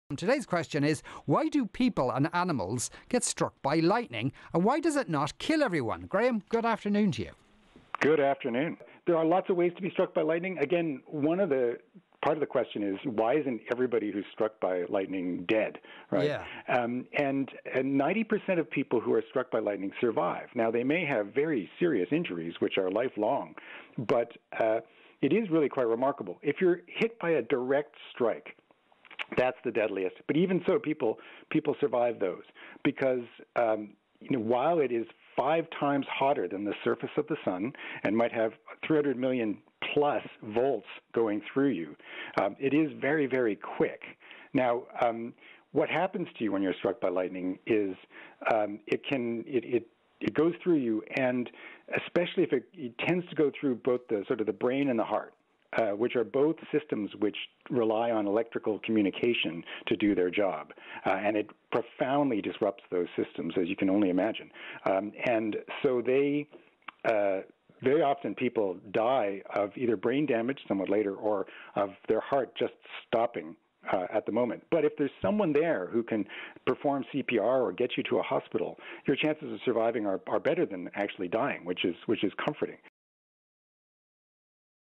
How do people survive being struck by lightning?
Dr. Graham Finlay, from the School of Politics and International Relations at University College Dublin, joined Sean for his weekly Tell Me why Slot.

This week he answered the question: Why do animals (and people) get struck by lightning and why does it not kill everyone?

Cover image: Lewis Whyld/PA Archive/PA Images